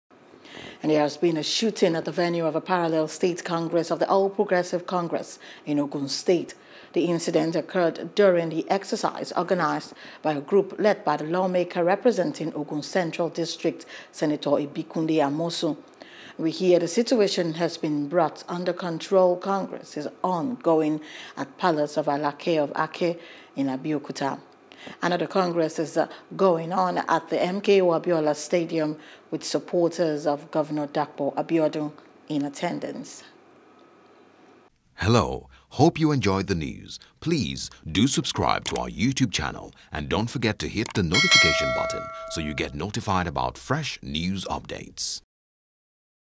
Politics: Shooting Reported At A.P.C Factional Congress In Ogun | NEWS
There has been a shooting at the venue of a parallel state congress of the All Progressives Congress in Ogun State.